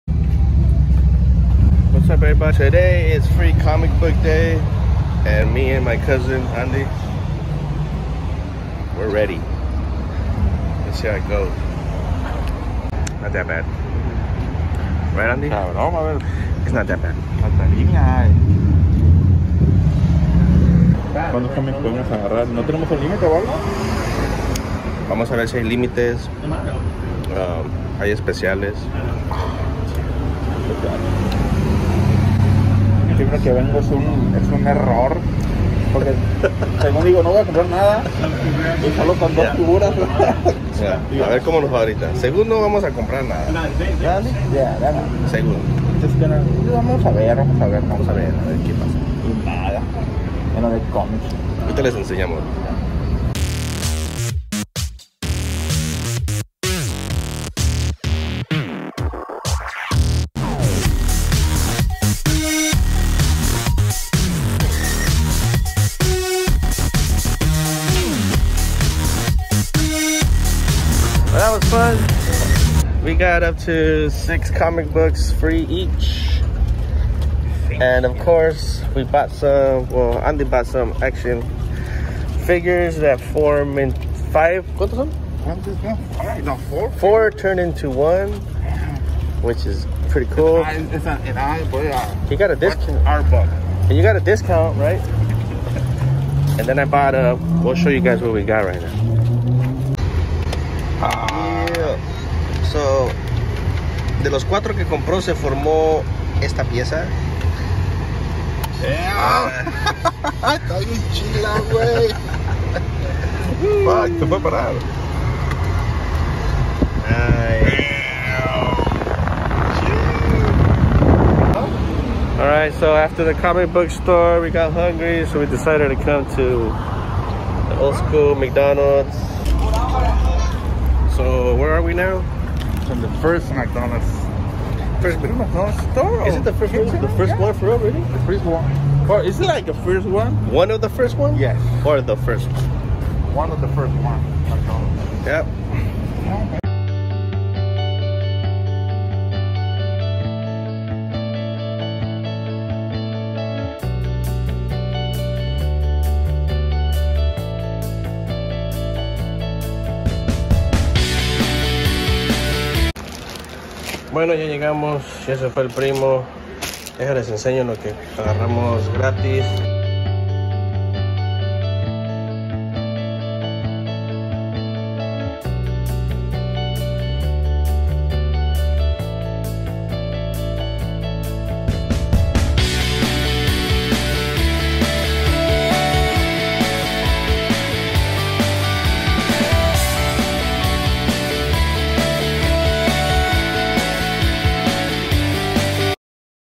Luis Jose Leyva Ep. 6 Free comic book day was fun in bellflower California.
Me and my Cuzin took advantage of Free comic book day.